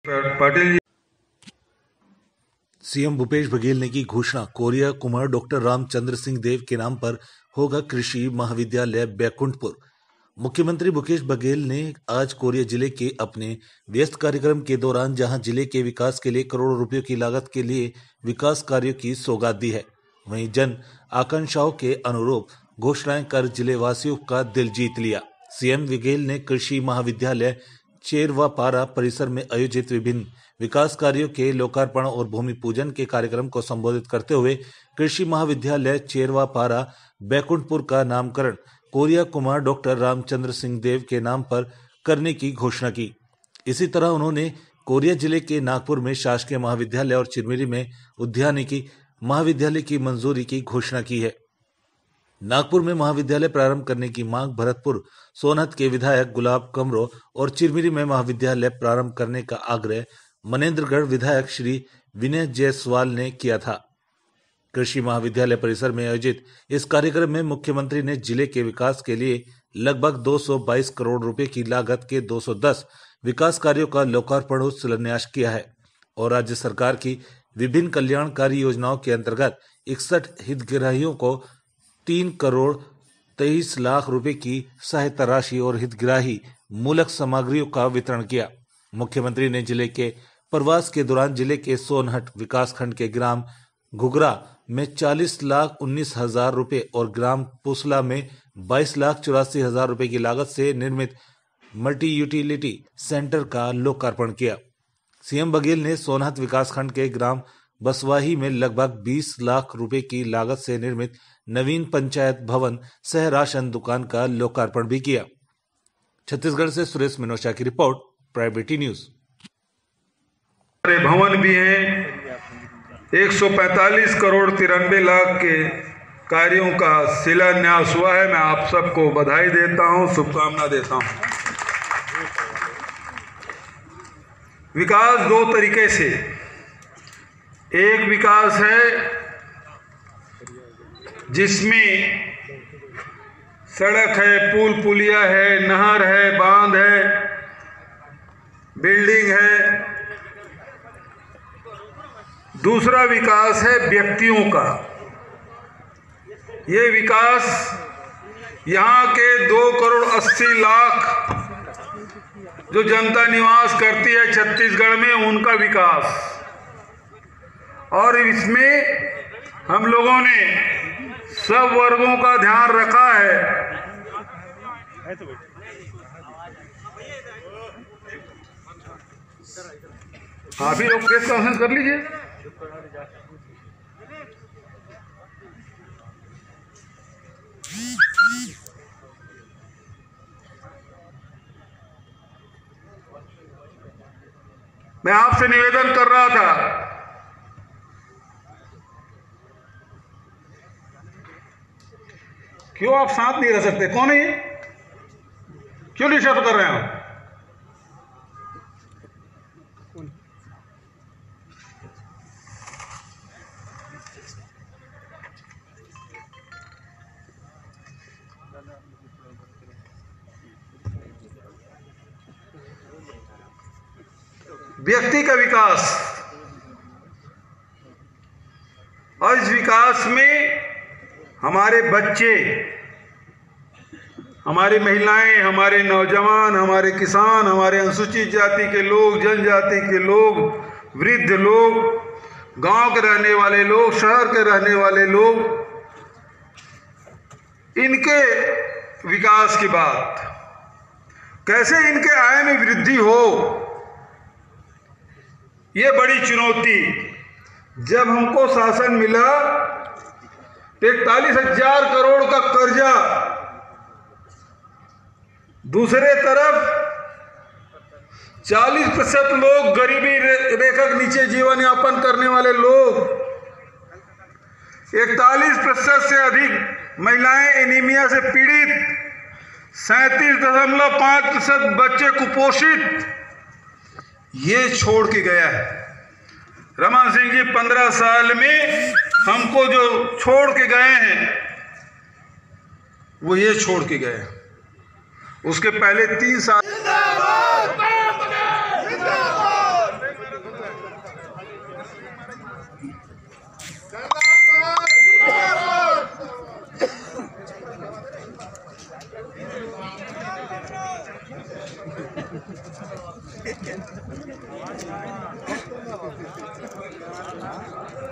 cmभूपेश बघेल ने की घोषणा कोरिया कुमार डॉरामचंद्र सिंहदेव के नाम पर होगा कृषि महाविद्यालय बैकुण्ठपुर
सीएम भूपेश बघेल ने की घोषणा...कोरिया कुमार डॉ. रामचंद्र सिंहदेव के नाम पर होगा कृषि महाविद्यालय बैकुण्ठपुर
मुख्यमंत्री भूपेश बघेल ने आज कोरिया जिले के अपने व्यस्त कार्यक्रम के दौरान जहां जिले के विकास के लिए करोड़ों रूपए की लागत के विकास कार्याें की सौगात दी।

वहीं जन आकांक्षाओं के अनुरूप घोषणाएं कर जिलेवासियों का दिल जीत लिया। सीएम बघेल ने कृषि महाविद्यालय चेरवापारा परिसर में आयोजित विभिन्न विकास कार्याें के लोकार्पण और भूमिपूजन के कार्यक्रम को संबोधित करते हुए कृषि महाविद्यालय चेरवापारा बैकुण्ठपुर का नामकरण कोरिया कुमार डॉ. रामचंद्र सिंहदेव के नाम पर करने की घोषणा की। इसी तरह उन्होंने कोरिया जिले के नागपुर में शासकीय महाविद्यालय और चिरमिरी में उद्यानिकी महाविद्यालय की मंजूरी की घोषणा की। नागपुर में महाविद्यालय प्रारंभ करने की मांग भरतपुर-सोनहत के विधायक गुलाब कमरो और चिरमिरी में महाविद्यालय प्रारंभ करने का आग्रह मनेन्द्रगढ़ विधायक श्री विनय जायसवाल ने किया था।
कृषि महाविद्यालय परिसर में आयोजित इस कार्यक्रम में मुख्यमंत्री ने जिले के विकास के लिए लगभग 222 करोड़ रूपए की लागत के 210 विकास कार्याें का लोकार्पण और शिलान्यास किया और राज्य सरकार की विभिन्न कल्याणकारी योजनाओं के अंतर्गत 61 हितग्राहियों को 3 करोड़ 23 लाख रूपए की सहायता राशि और हितग्राही मूलक सामग्रियों का वितरण किया। मुख्यमंत्री ने जिले के प्रवास के दौरान जिले के सोनहट विकासखण्ड के ग्राम घुघरा में 40 लाख 19 हजार रूपए और ग्राम पुसला में 22 लाख 84 हजार रूपए की लागत से निर्मित मल्टीयूटीलिटी सेंटर का लोकार्पण किया। सीएम बघेल ने सोनहत विकासखण्ड के ग्राम बसवाही में लगभग 20 लाख रूपए की लागत से निर्मित नवीन पंचायत भवन सह-राशन दुकान का लोकार्पण भी किया।
छतीसगढ़ से सुरेश मिनोचा की रिपोर्ट